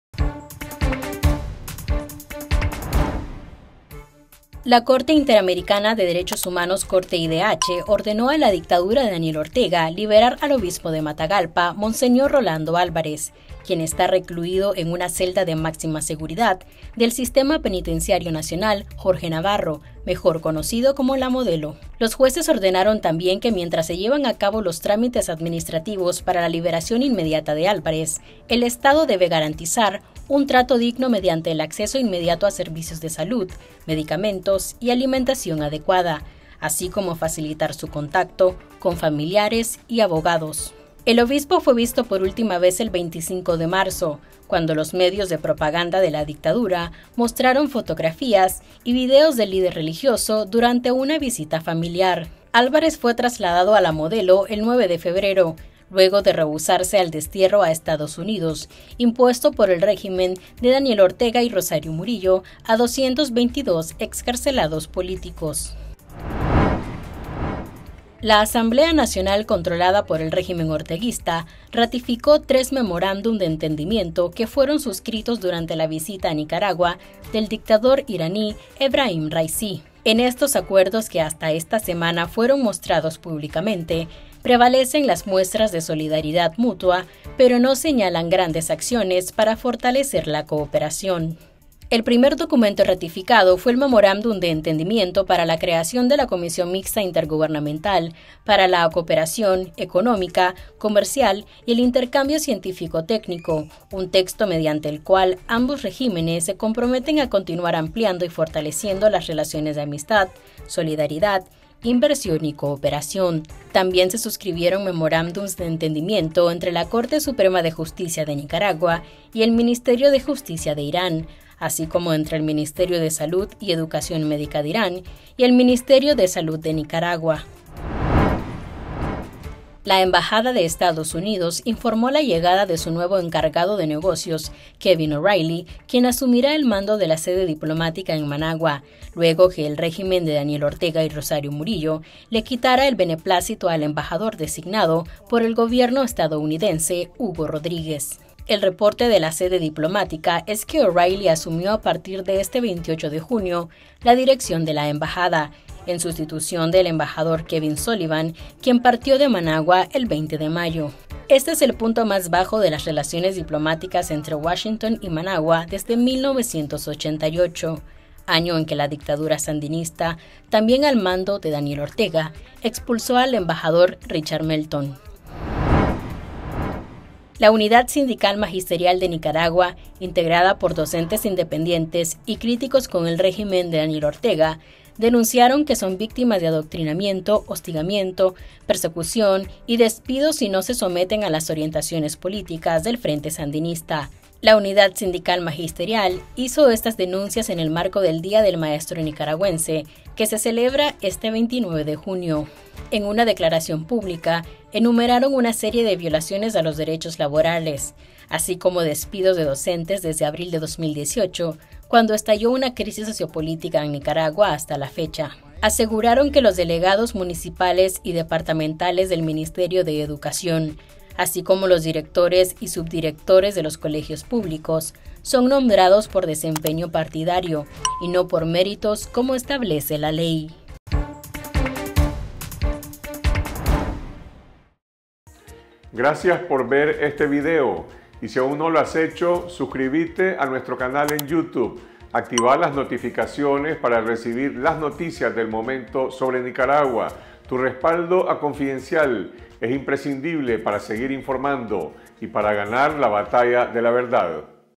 Corte IDH ordena la liberación de obispo Rolando Álvarez
La Corte Interamericana de Derechos Humanos (Corte IDH) ordenó a la dictadura de Daniel Ortega liberar al obispo de Matagalpa, monseñor Rolando Álvarez, quien está recluido en una celda de máxima seguridad del Sistema Penitenciario Nacional Jorge Navarro, mejor conocido como “La Modelo”.
Los jueces ordenaron también que, mientras se llevan a cabo los trámites administrativos para la liberación inmediata de Álvarez, el Estado debe “garantizar un trato digno mediante el acceso inmediato a servicios de salud, medicamentos y alimentación adecuada, así como facilitar su contacto con familiares y abogados”.
El obispo fue visto por última vez el 25 de marzo, cuando los medios de propaganda de la dictadura mostraron fotografías y videos del líder religioso, durante una visita familiar.
Álvarez fue trasladado a La Modelo el 9 de febrero, luego de rehusarse al destierro a Estados Unidos impuesto por el régimen de Daniel Ortega y Rosario Murillo a 222 excarcelados políticos.

Estas son otras noticias destacadas de la jornada:

- Dictadura e Irán rellenan acuerdo con muestras de solidaridad.
- Embajada de EE.UU. anuncia la llegada de su encargado de negocios.
- Maestros nicaragüenses denuncian adoctrinamiento, persecución y despidos.